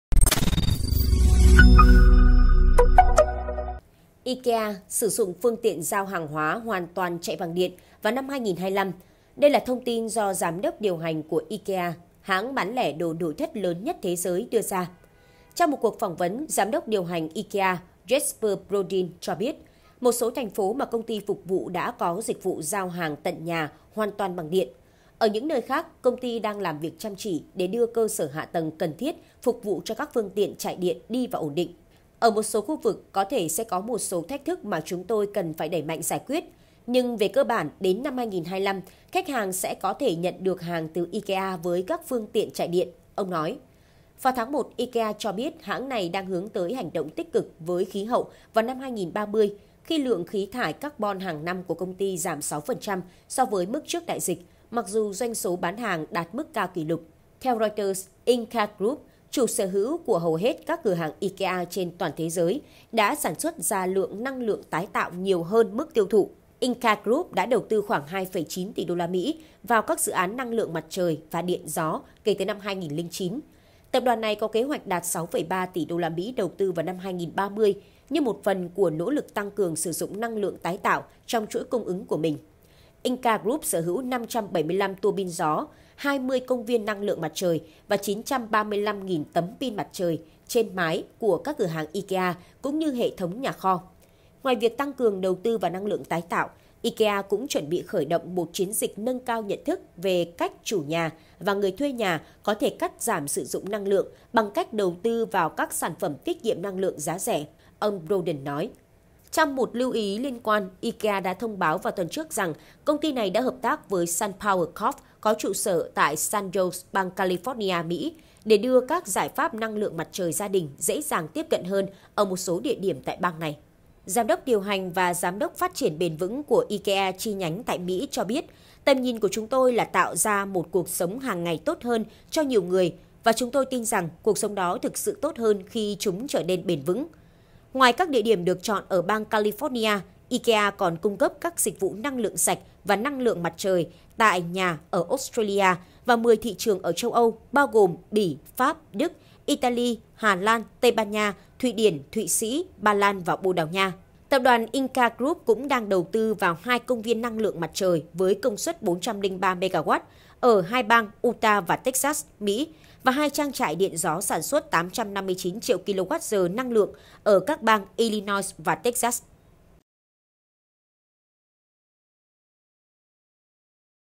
IKEA SỬ DỤNG PHƯƠNG TIỆN GIAO HÀNG HOÀN TOÀN CHẠY BẰNG ĐIỆN VÀO NĂM 2025
IKEA sử dụng phương tiện giao hàng hoàn toàn chạy bằng điện vào năm 2025. Đây là thông tin do Giám đốc điều hành của IKEA, hãng bán lẻ đồ nội thất lớn nhất thế giới đưa ra. Trong một cuộc phỏng vấn, Giám đốc điều hành IKEA Jesper Brodin cho biết, một số thành phố mà công ty phục vụ đã có dịch vụ giao hàng tận nhà hoàn toàn bằng điện. Ở những nơi khác, công ty đang làm việc chăm chỉ để đưa cơ sở hạ tầng cần thiết phục vụ cho các phương tiện chạy điện đi vào ổn định.
"Ở một số khu vực, có thể sẽ có một số thách thức mà chúng tôi cần phải đẩy mạnh giải quyết. Nhưng về cơ bản, đến năm 2025, khách hàng sẽ có thể nhận được hàng từ IKEA với các phương tiện chạy điện", ông nói.
Vào tháng 1, IKEA cho biết, hãng này đang hướng tới hành động tích cực với khí hậu vào năm 2030, khi lượng khí thải carbon hàng năm của công ty giảm 6% so với mức trước đại dịch mặc dù doanh số bán hàng đạt mức cao kỷ lục.
Theo Reuters, Ingka Group, chủ sở hữu của hầu hết các cửa hàng IKEA trên toàn thế giới, đã sản xuất ra lượng năng lượng tái tạo nhiều hơn mức tiêu thụ. Ingka Group đã đầu tư khoảng 2,9 tỷ USD vào các dự án năng lượng mặt trời và điện gió kể từ năm 2009.
Tập đoàn này có kế hoạch đạt 6,3 tỷ USD đầu tư vào năm 2030 như một phần của nỗ lực tăng cường sử dụng năng lượng tái tạo trong chuỗi cung ứng của mình. Ingka Group sở hữu 575 turbine gió, 20 công viên năng lượng mặt trời và 935.000 tấm pin mặt trời trên mái của các cửa hàng IKEA cũng như hệ thống nhà kho.
Ngoài việc tăng cường đầu tư vào năng lượng tái tạo, IKEA cũng chuẩn bị khởi động một chiến dịch nâng cao nhận thức về cách chủ nhà và người thuê nhà có thể cắt giảm sử dụng năng lượng bằng cách đầu tư vào các sản phẩm tiết kiệm năng lư ợng giá rẻ, ông Broden nói.
Trong một lưu ý liên quan, IKEA đã thông báo vào tuần trước rằng công ty này đã hợp tác với SunPower Corp., có trụ sở tại San Jose, bang California, Mỹ, để đưa các giải pháp năng lượng mặt trời gia đình dễ dàng tiếp cận hơn ở một số địa điểm tại bang này.
Giám đốc điều hành và Giám đốc phát triển bền vững của IKEA chi nhánh tại Mỹ, cho biết: "Tầm nhìn của chúng tôi là tạo ra một cuộc sống hàng ngày tốt hơn cho nhiều người và chúng tôi tin rằng cuộc sống đó thực sự tốt hơn khi chúng trở nên bền vững".
Ngoài các địa điểm được chọn ở bang California, IKEA còn cung cấp các dịch vụ năng lượng sạch và năng lượng mặt trời tại nhà ở Australia và 10 thị trường ở châu  u bao gồm Bỉ, Pháp, Đức, Italy, Hà Lan, Tây Ban Nha, Thụy Điển, Thụy Sĩ, Ba Lan và Bồ Đào Nha. Tập đoàn Ingka Group cũng đang đầu tư vào hai công viên năng lượng mặt trời với công suất 403 MW ở hai bang Utah và Texas, Mỹ và hai trang trại điện gió sản xuất 859 triệu kWh năng lượng ở các bang Illinois và Texas.